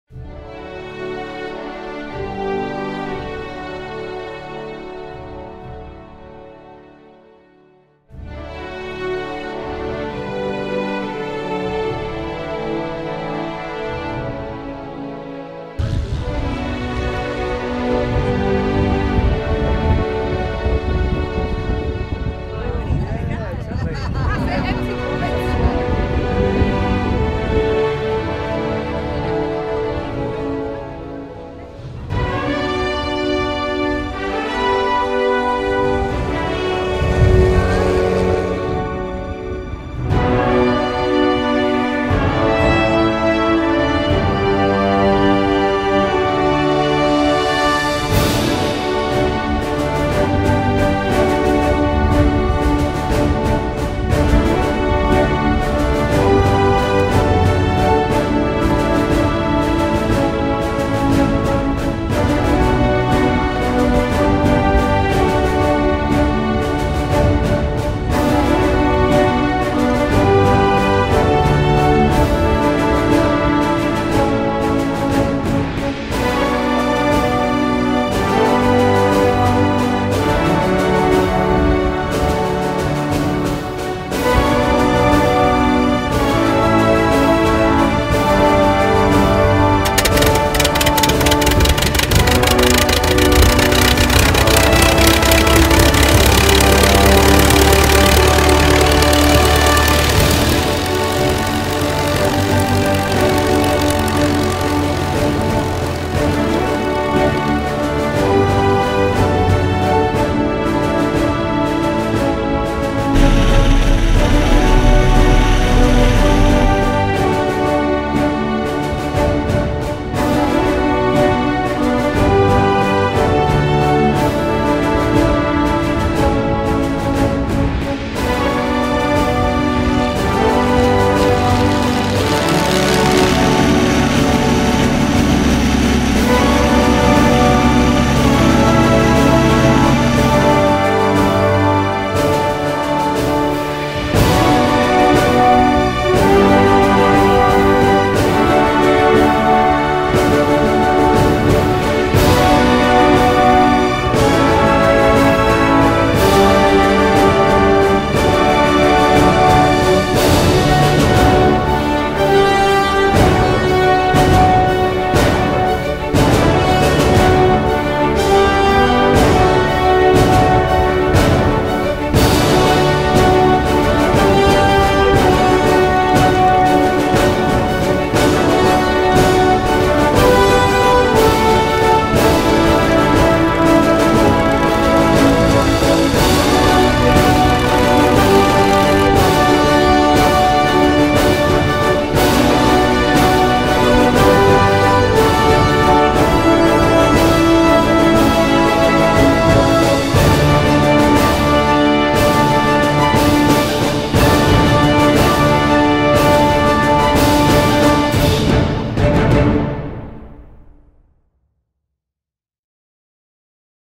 Dad's Tiger Moth Flight at Henstridge Airfield. 1st August 2015
To celebrate his 83rd birthday Dad enjoyed a flight in a 1935 Tiger Moth - DH 82 A - BB803 G-ADWJ, piloted by Annabelle Burroughes.